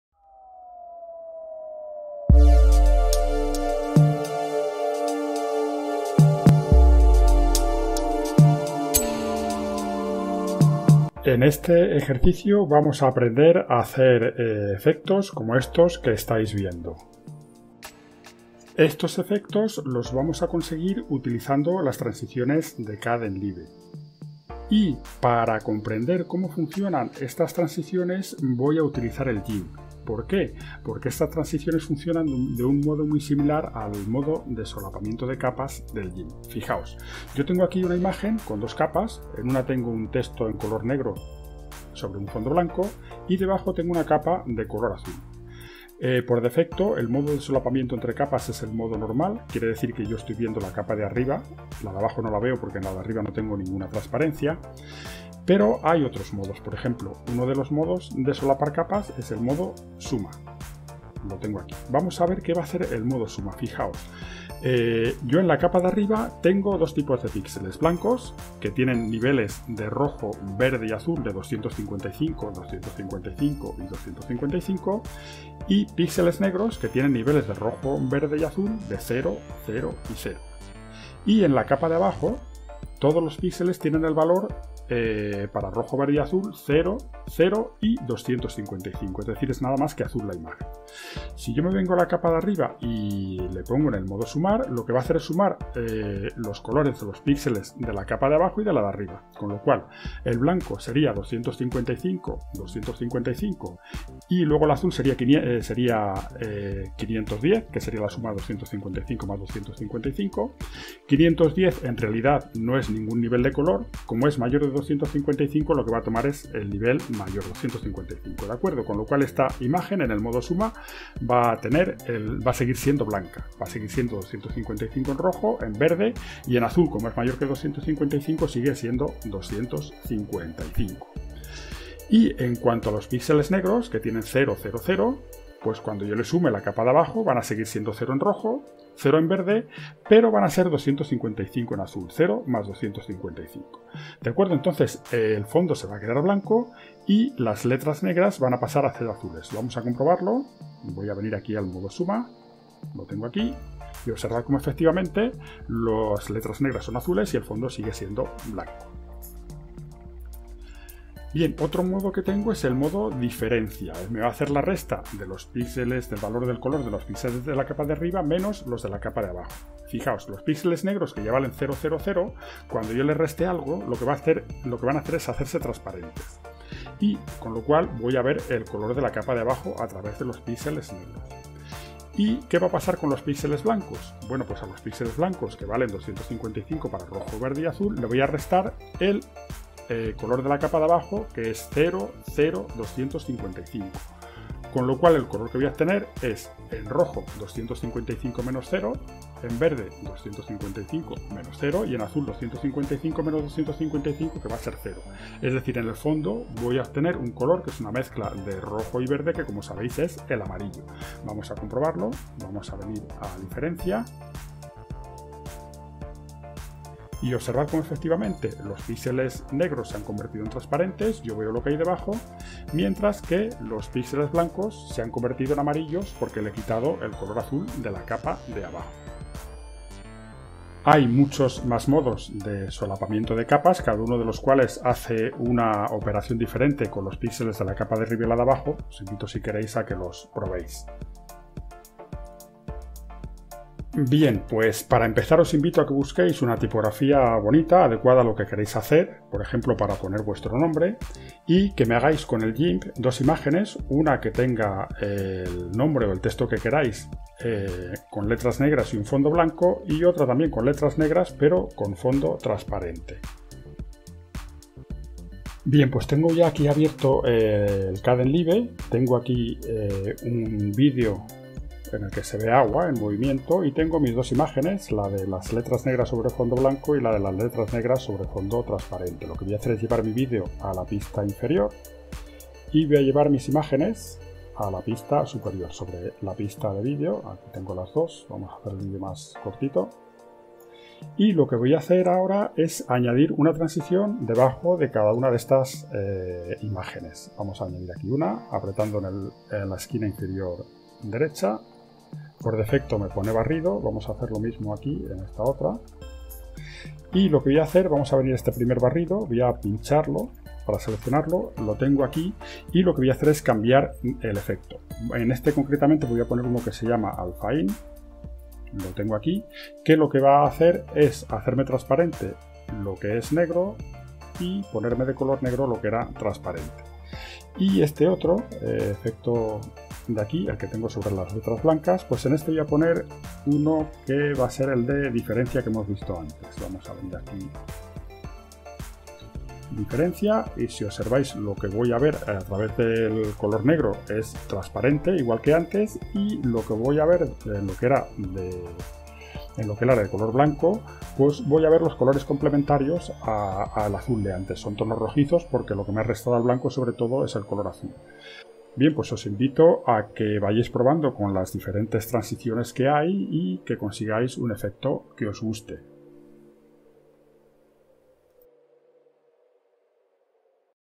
Kdenlive transiciones
Uso de las transiciones de kdenlive para obtener diferentes efectos.